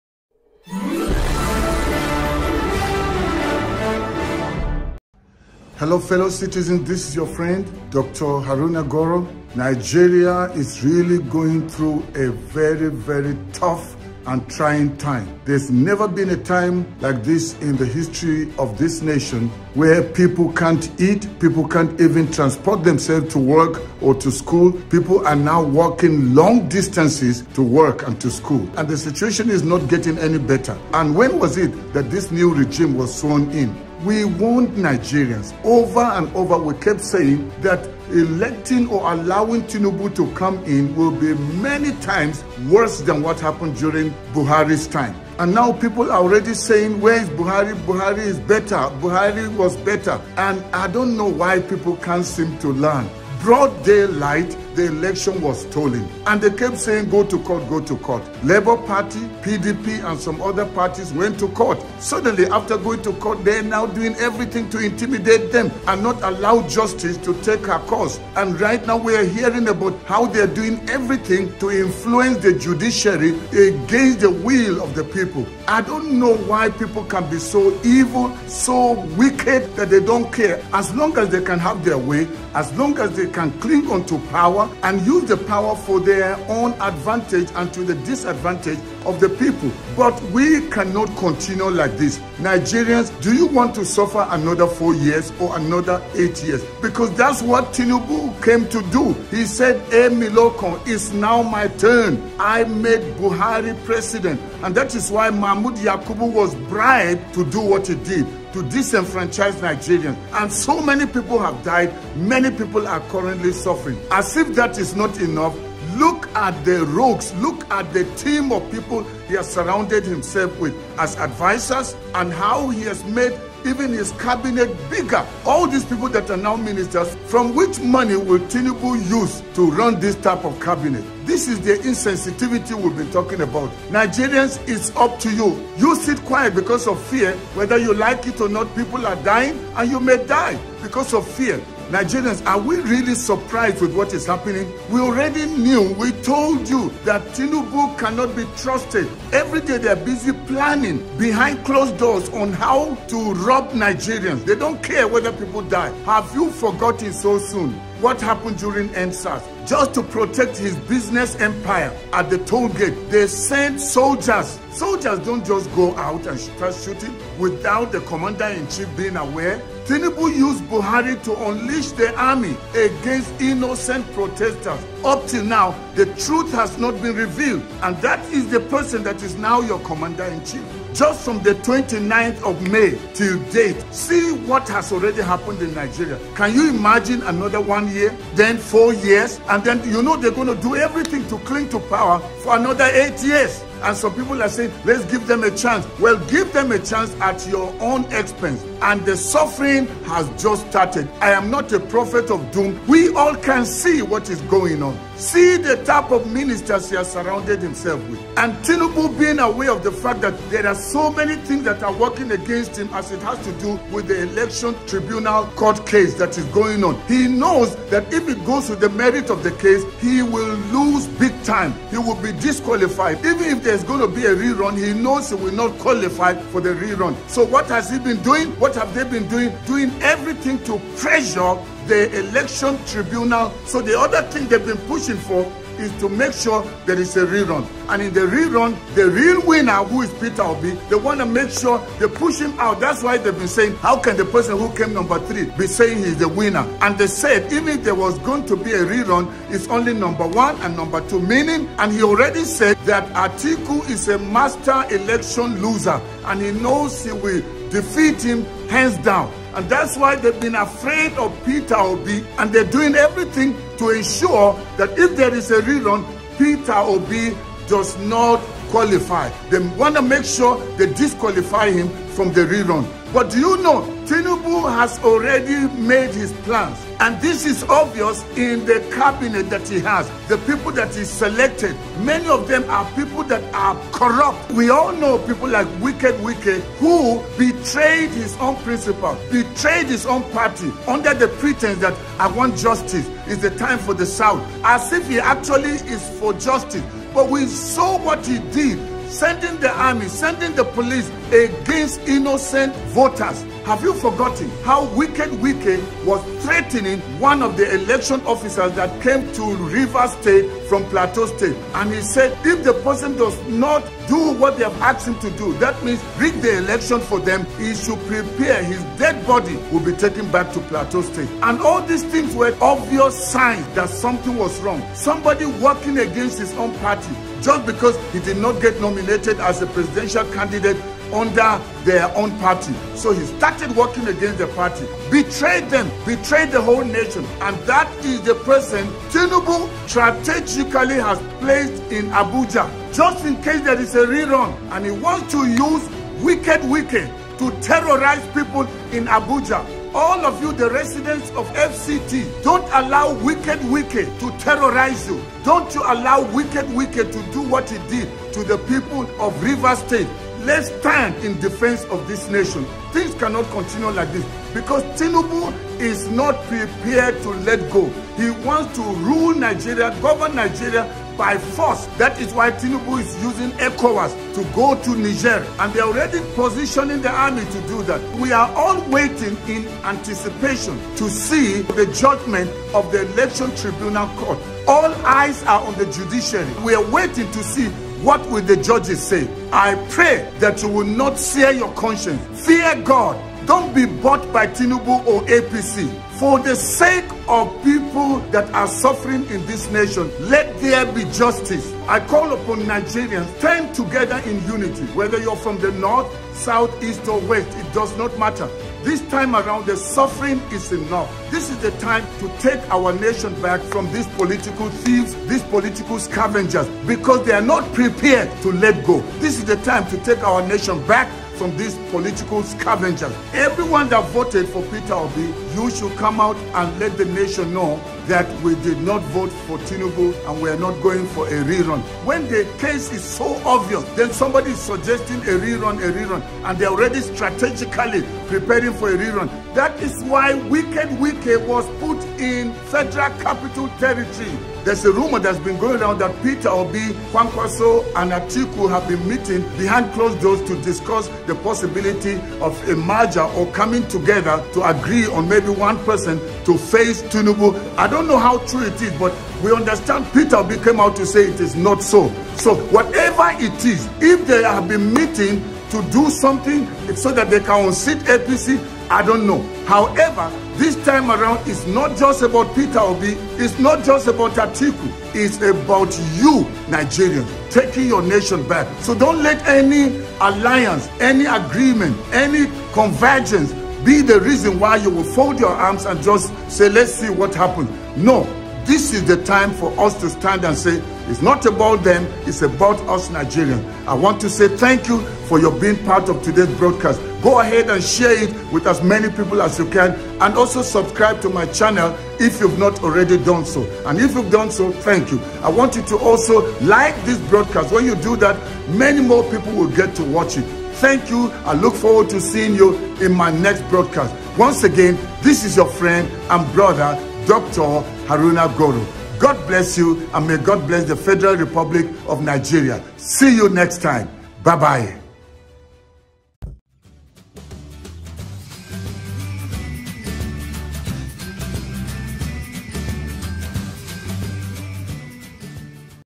The Judiciary Is Been Influenced Against The Hope Of The People.
Atiku Is A Master Election Looser,
Which Is Why These Thieves Can Not Stay.

This Is Our New BVI Channel 1
After Our Main Channel Was Taken Down